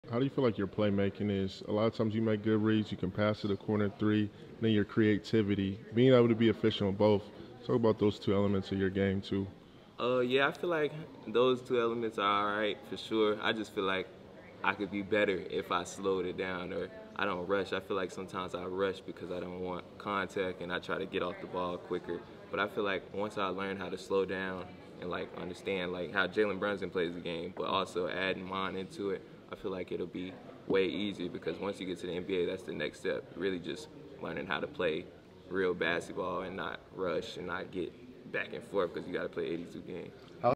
Rob Dillingham talks playing with pace and not rushing his game!!
Rob Dillingham talks about playing with pace and learning to not rush his moves. He mentions guys like Jalen Brunson and how he can incorporate his game into his own!! 🔥